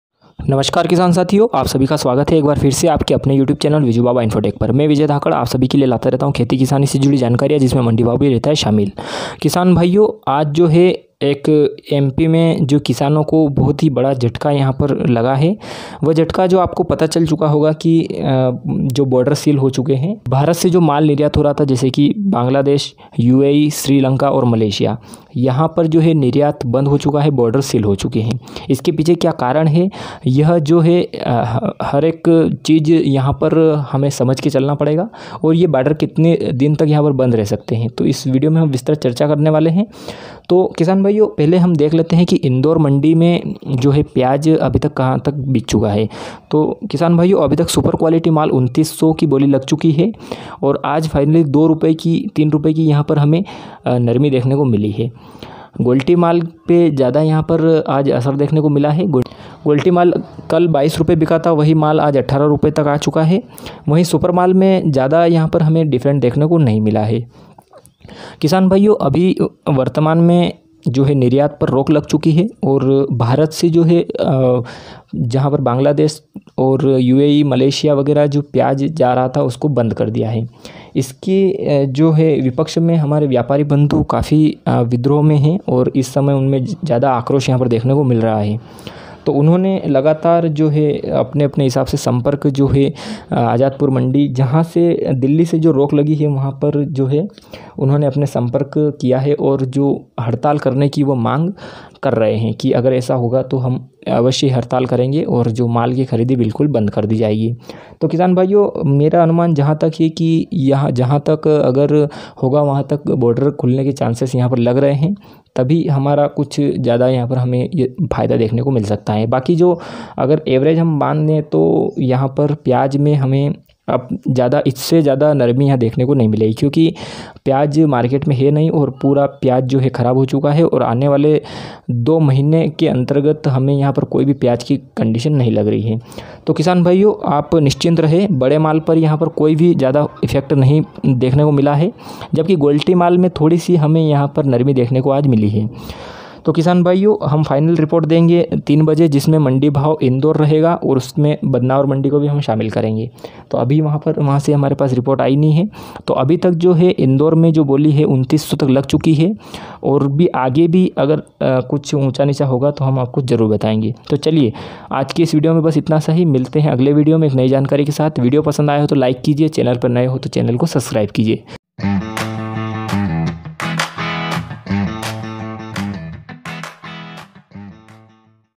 इंदौर मण्डी प्याज रेट|निर्यात से कितना असर पड़ा|ऊपर में 29 रुपये बिका|गोलटी माल की क्या रही हालत🤔🤔
इंदौर मण्डी प्याज रेट|निर्यात से कितना असर पड़ा|ऊपर में 29 रुपये बिका|गोलटी माल की क्या रही पोजीसन